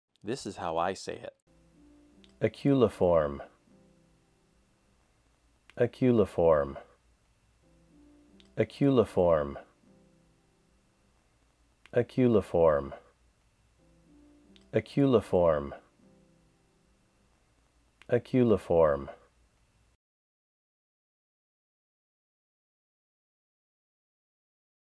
This is How I Say...aculeiform
How do you say aculeiform? I have started a project to not only let you hear how to say aculeiform and all other words in the dictionary, I’m also SHOWING you how to say aculeiform.